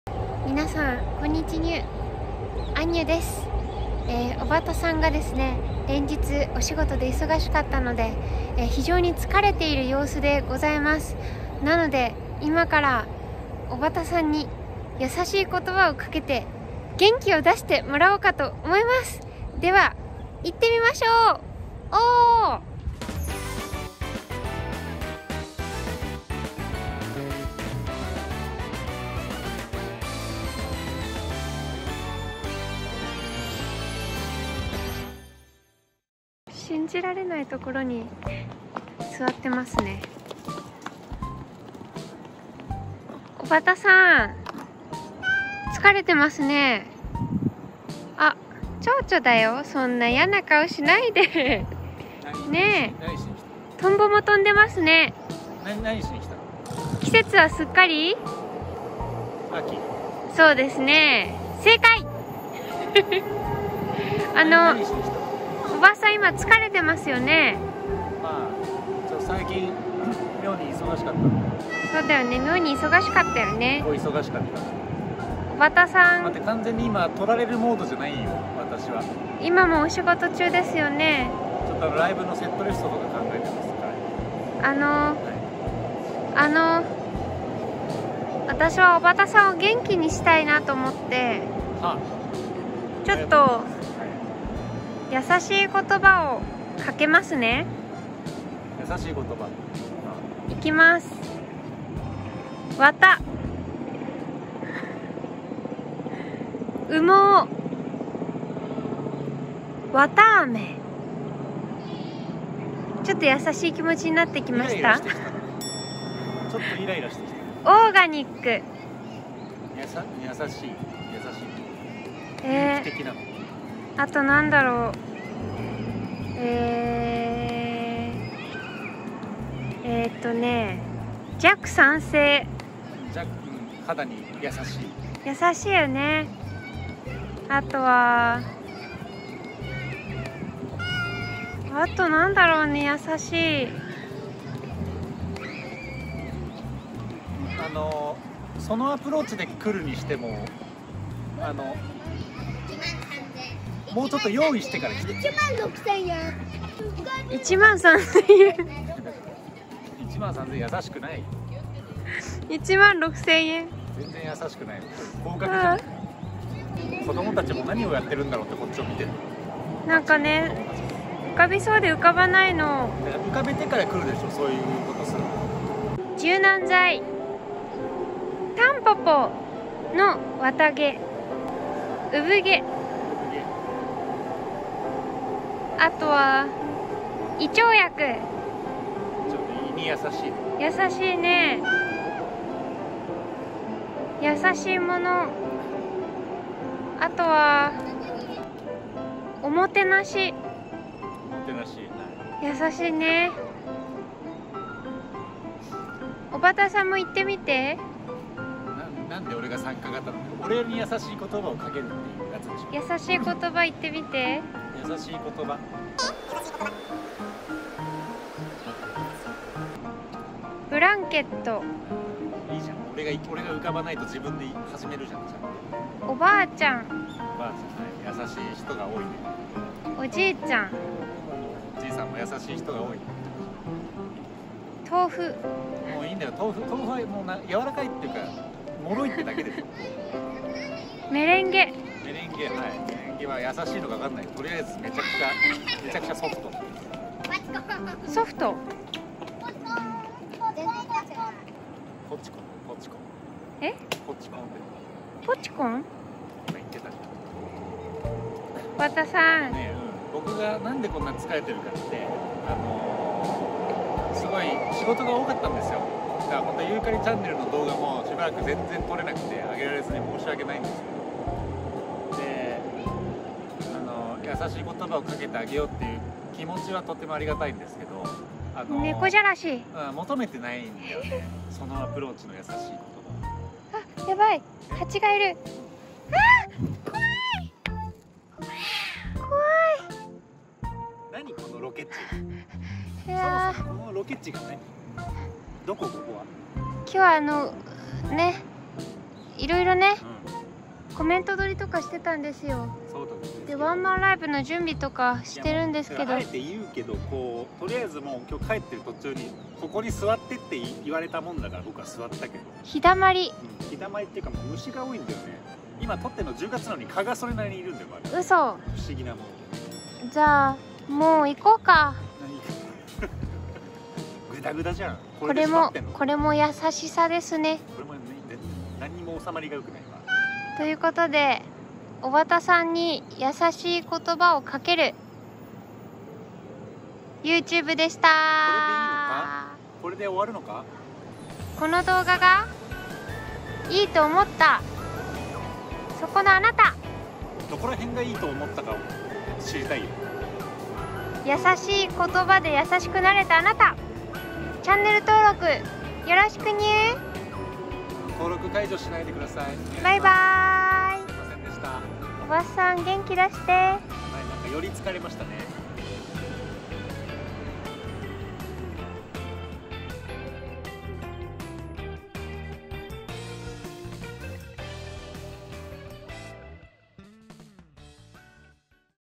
【思いやり企画】疲れてる小幡さんに優しい言葉をかけてあげよう！
久々の投稿になりすみません。
連日お仕事に追われ、動画を編集する暇もなかったオバタ。

どこから見ても表情が疲れているオバタを見かねたあんにゅが
「優しい言葉をかけてあげよう！」と
突発的に思い立った企画動画。

男女二人組音楽ユニット「コアラモード．」
新しくYouTubeチャンネルをはじめましたよ。
以後お見知り置きを！

出演：あんにゅ（企画）
出演：小幡康裕(BGM&編集)